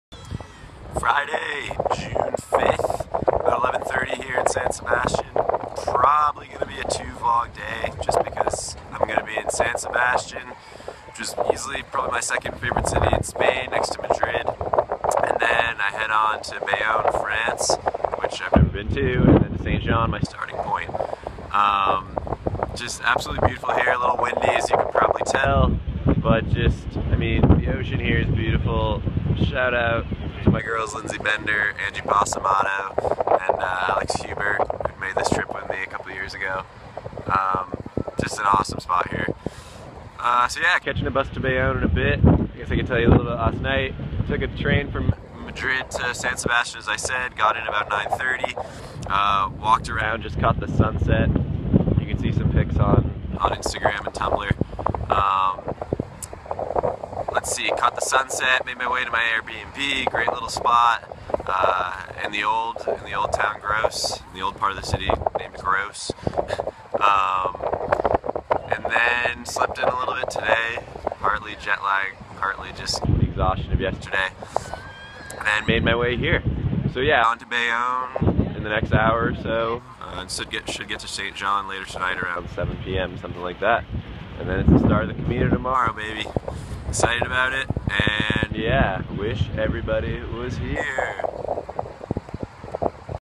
Bot's Camino Vlog 3.1: San Seb 5ever
Bit windy but beautiful morning in San Sebastián!   Didn't get a chance to swim because I woke up later than I wanted to but whatever! Enjoy!